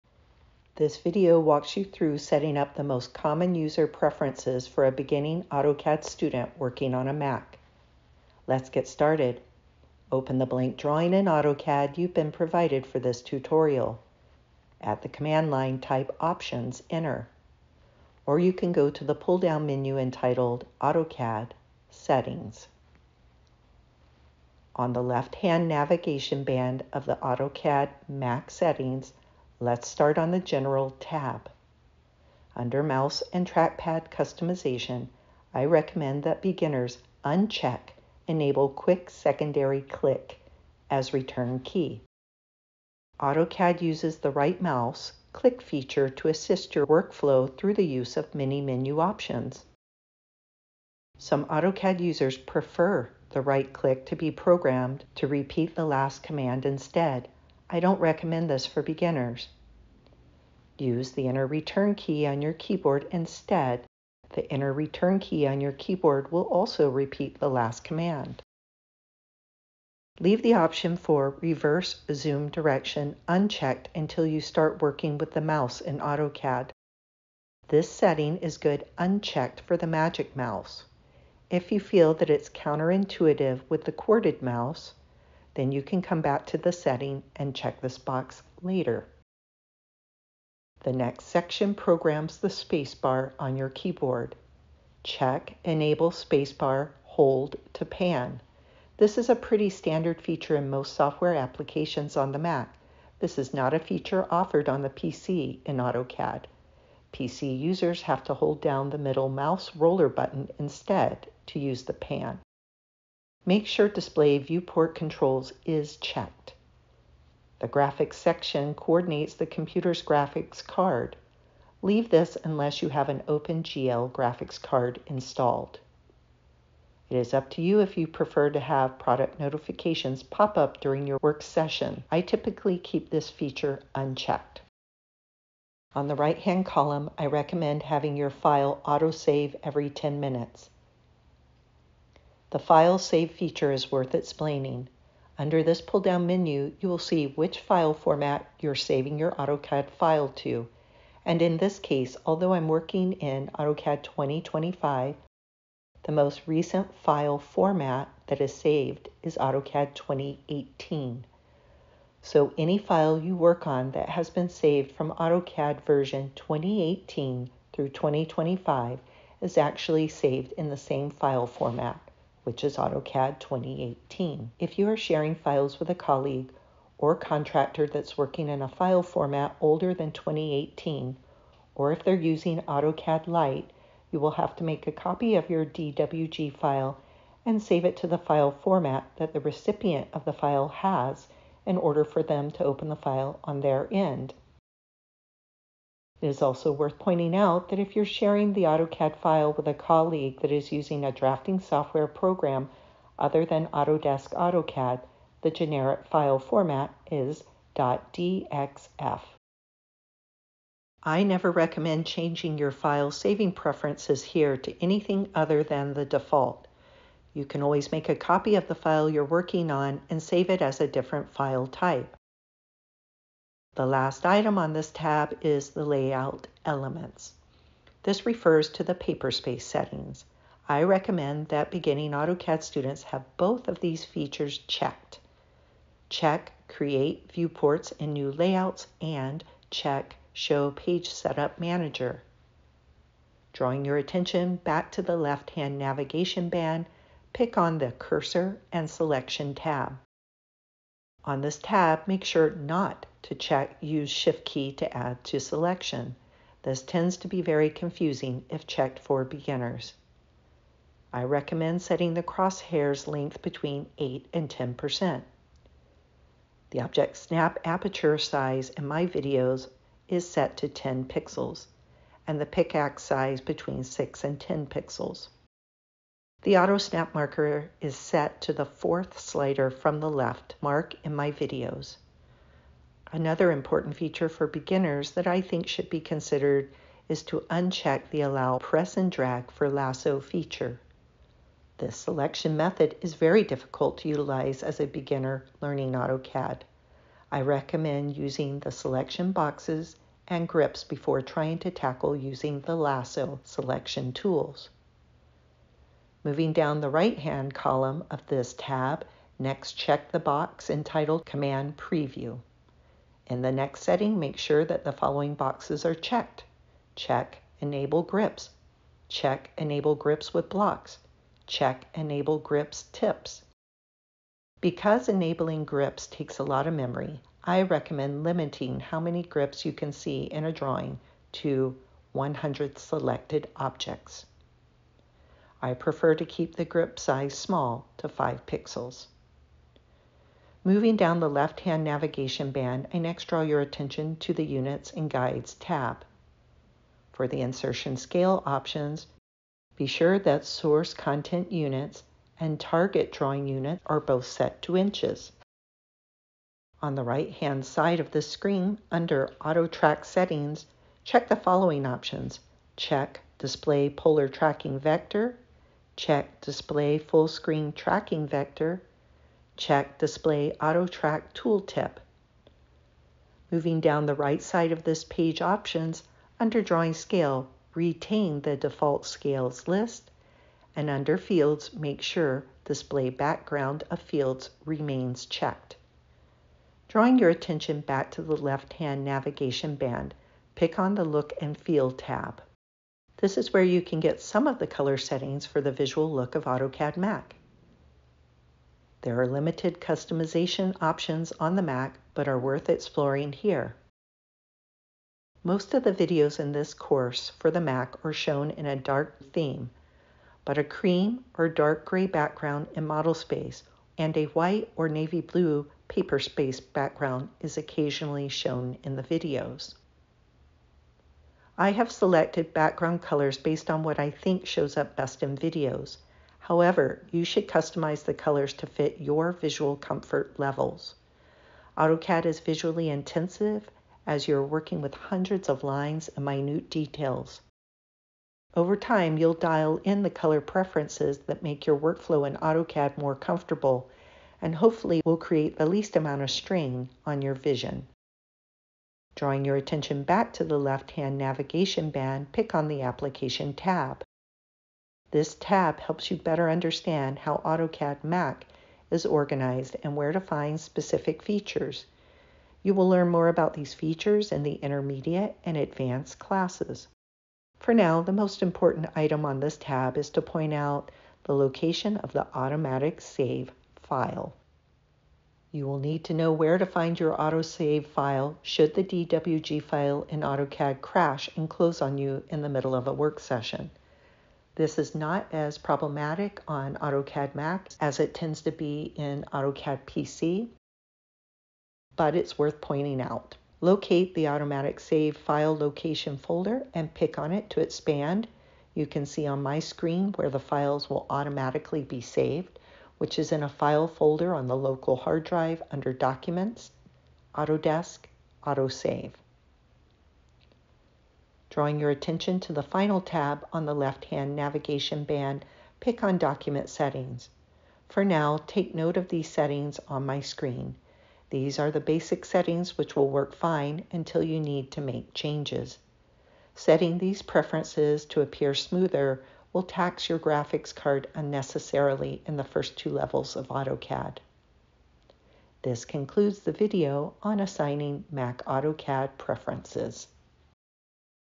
Assigning Mac AutoCAD Preferences
This video is intended for the beginning AutoCAD setting user preference on a Mac for architecture - related industries in the United States. This video is tailored for students to be able to follow along with the AutoCAD settings for videos on this YouTube site.

Topics include: AutoCAD Mac preferences, Options command, Drafting tab, Selection tool choices, AutoSNAP, AutoTrack, User Preferences tab, Right-click customization, right-mouse click option, Space Bar keyboard customization, Default units of measurement for insertion, Associative dimensioning, Lineweight settings (inches), Default Scale List (Imperial), Block Editor Settings, Selection tab: Enable all grips choices, uncheck "Allow Press and Drag Lasso" (just for the beginning weeks of using AutoCAD), all color preferences for the display window and graphic interface, Files tab: location of AutoSAVE files (temp folder location), Open and Save Tab: File format type, Automatic Save setting timeframe, File extension for temporary files.